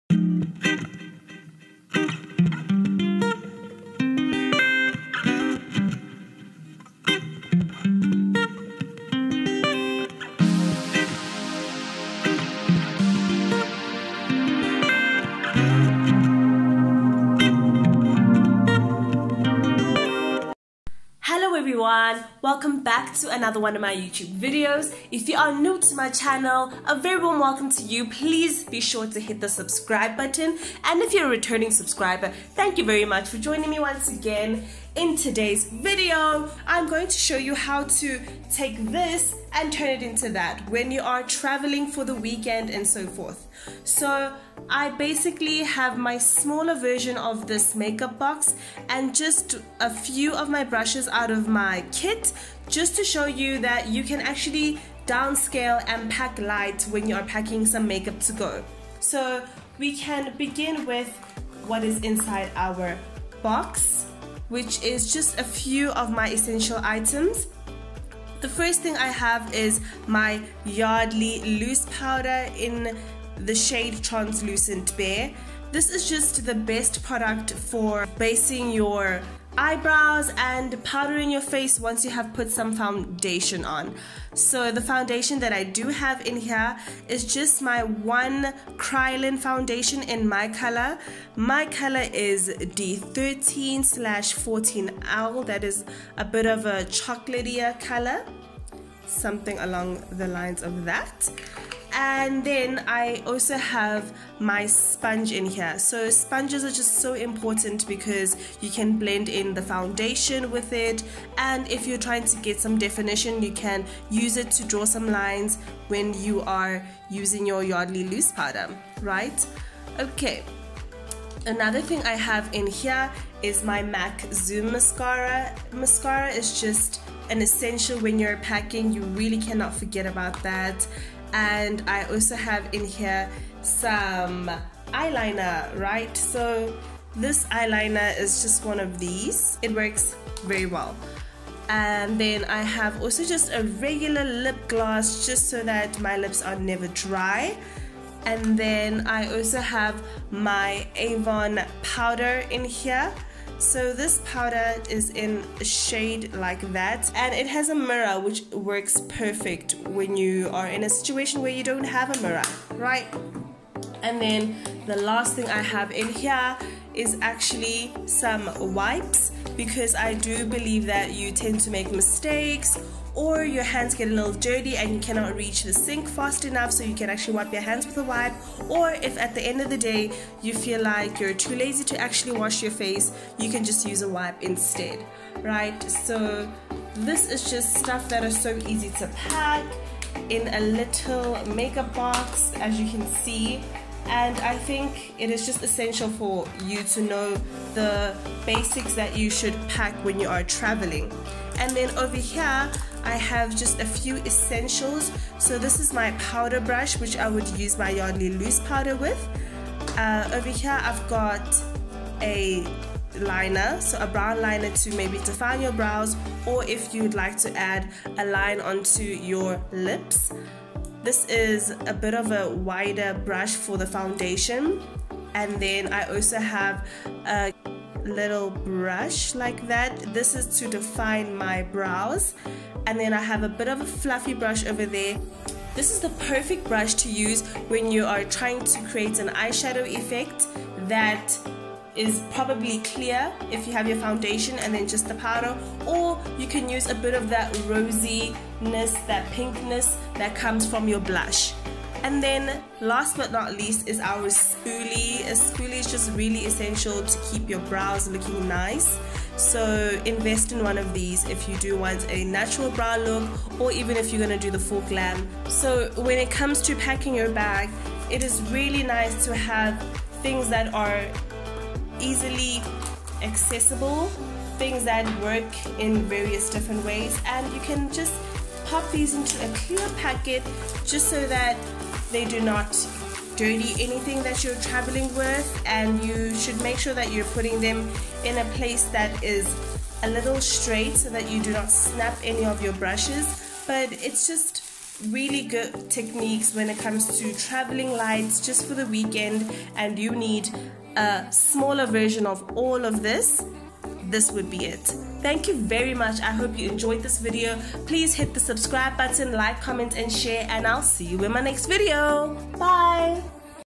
MAKEUP BAG ESSENTIALS | HOW TO PACK LIGHT WHEN YOU TRAVEL | SOUTH AFRICAN BEAUTY BLOGGER
Hello Everyone!

THESE ARE THE ONLY MAKEUP PRODUCTS NEEDED IN YOUR TRAVEL BAG.
I unpacked my makeup bag to show you the most essential products necessary for travelling.

Products Used -
Avon Blush
Black Eyeliner
Brown Eyeliner pencil
Kryolan -Derma Color - D13/14L
Lip gloss
M.A.C Zoom Mascara -
Yardley Loose Powder - Translucent Bare

Tools Used-
Blending Sponge
Flat Foundation Brush - Medium
Fluffy Powder Brush - Medium
Fluffy Eyeshadow Brush - small
Spooly

South African Beauty Blogger
Enjoy! xx